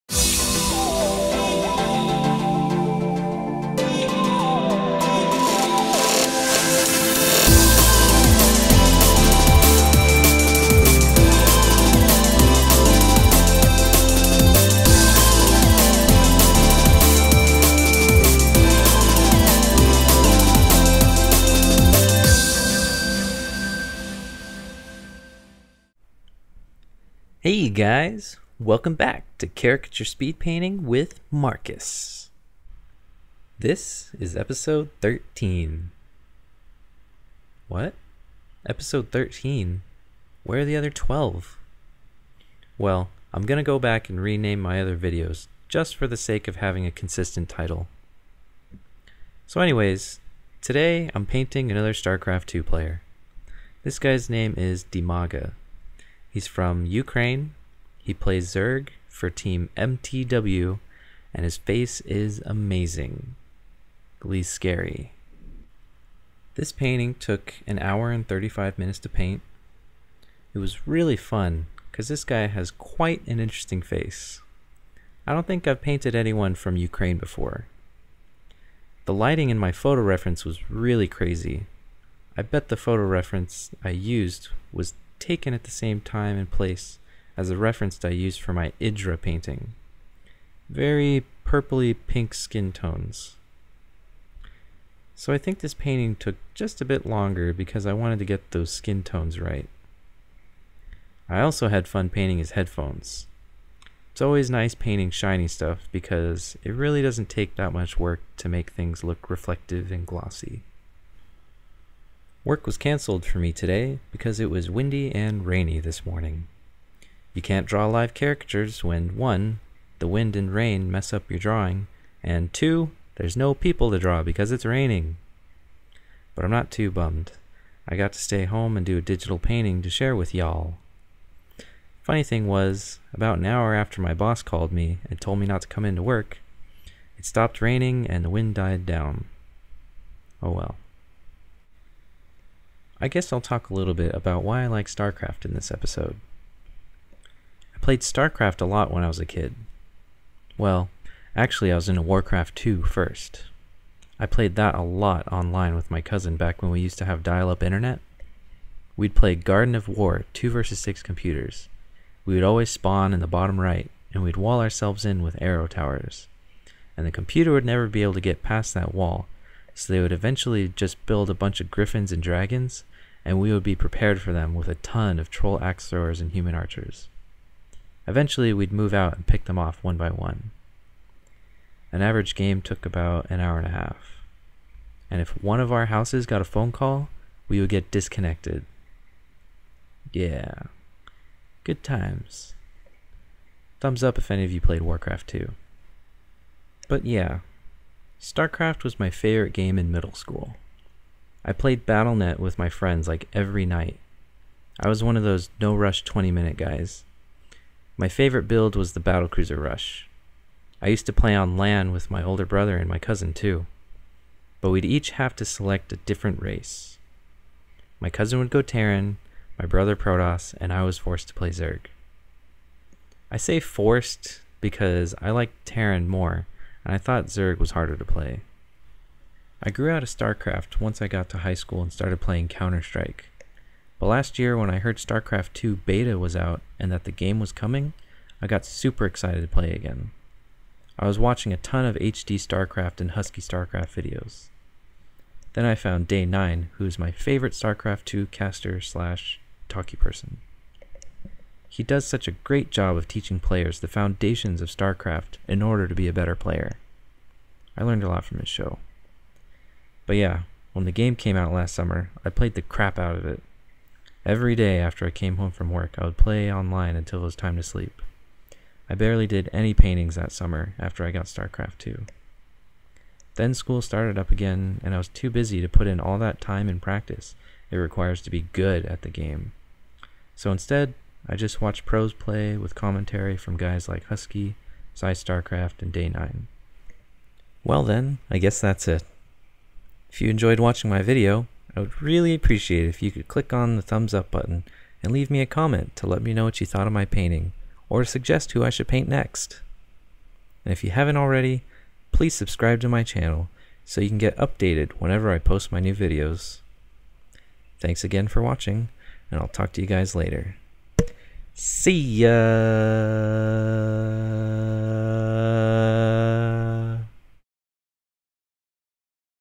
Caricature Speedpainting with Marcus Episode 13: Dimaga
I do a painting of starcraft2 player Dimaga!  I also talk a bit about my history with starcraft.